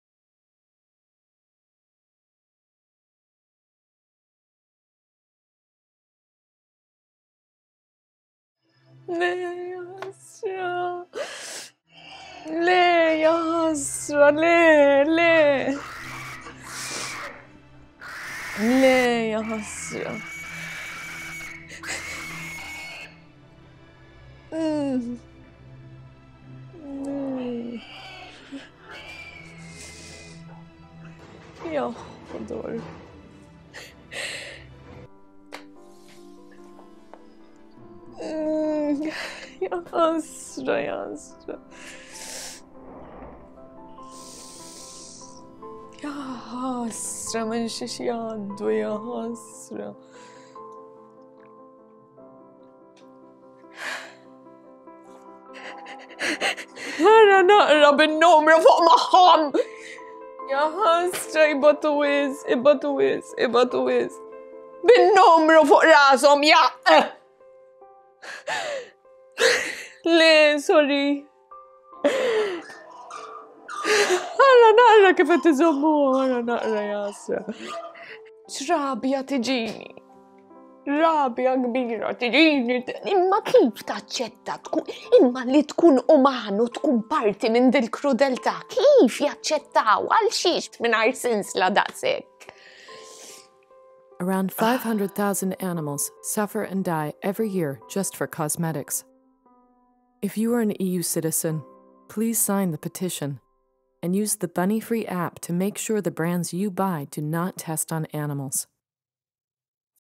My Experience Moira Delia, MSPCA
TARGET REACHED!
 1,002,757 signatures collected.
The EU can be animal testing free.
Please watch this video and if you feel inspired to create change, please sign the petition and ask your friends to do so too so that we can hammer in our position on this matter.

Thank YOU to MSPCA, PETA (People for the Ethical Treatment of Animals), Fabio Spiteri, Nate Darmanin, Valentina Rossi, Moira Delia, Frank Zammit and Clare Agius for all your assistance.
Special thank to Matthew Scibilia for helping with this shoot.

It is important to keep updated with the process and progress of the European Citizens' Initiative (ECI) on this issue and hope that change is done soon.